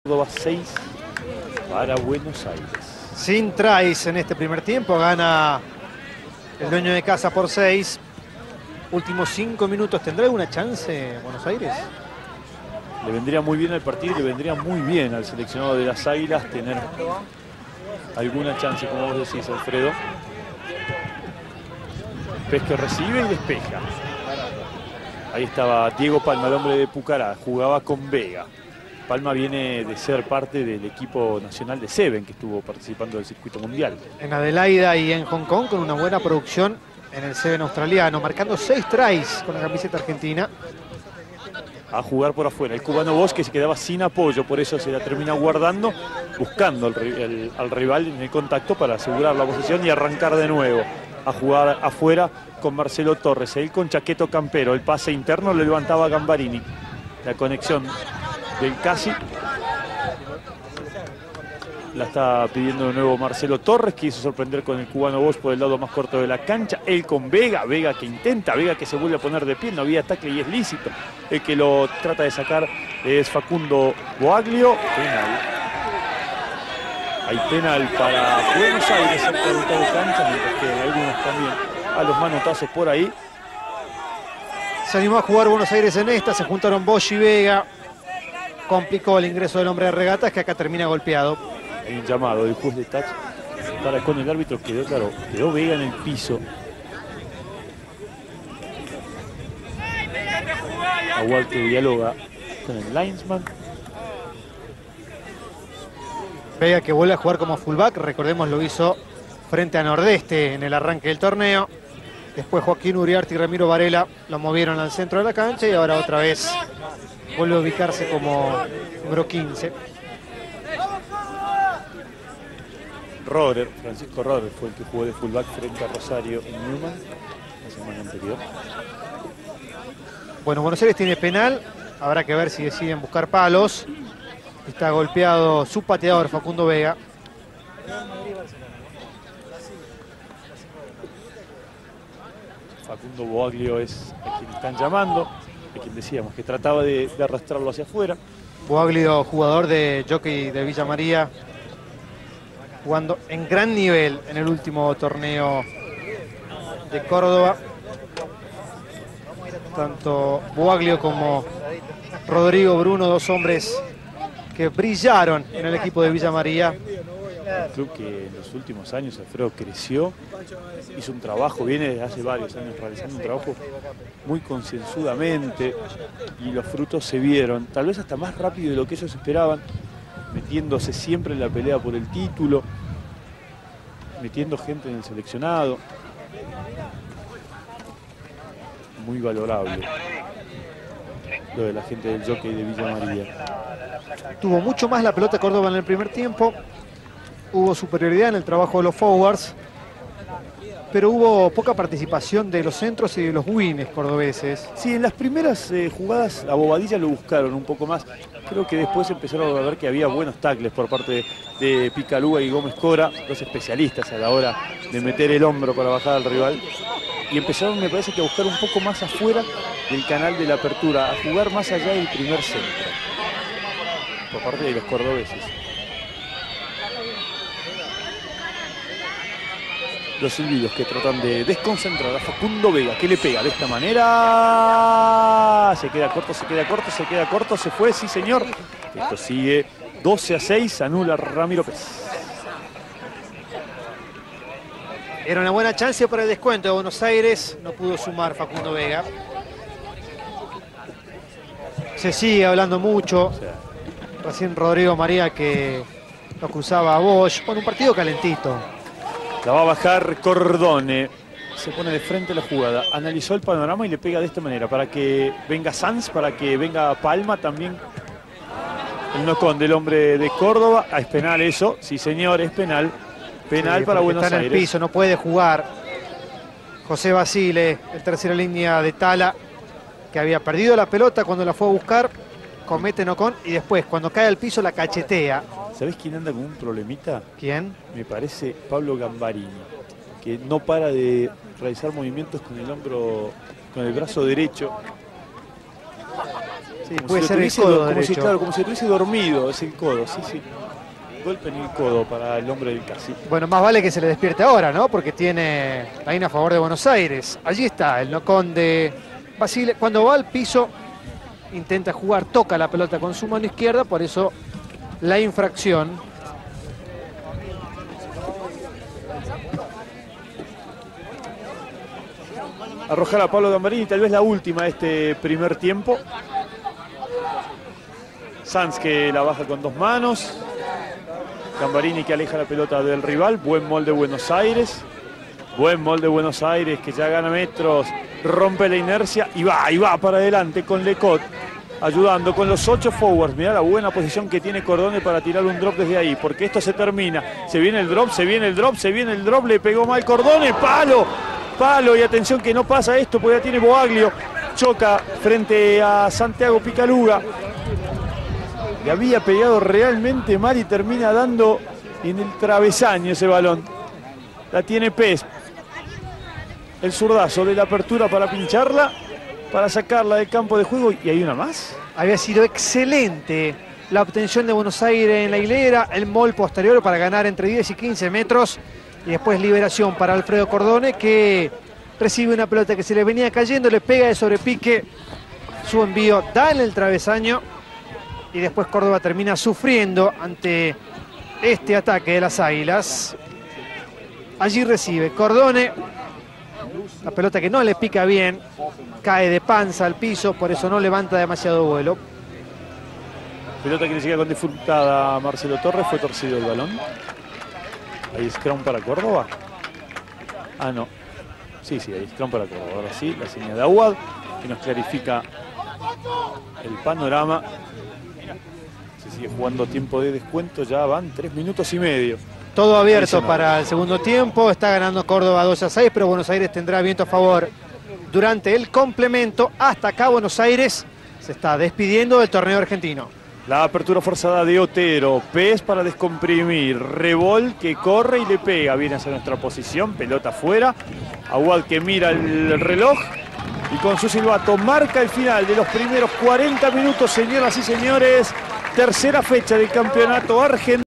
GASTON CORTES vs. Buenos Aires [Parte 05]
Partido completo de Gastón Cortés, primera línea del Córdoba Athletic Club y el Seleccionado de Córdoba, por la fase de grupos del Campeonato Argentino de Mayores 2010 frente al Seleccionado de Buenos Aires.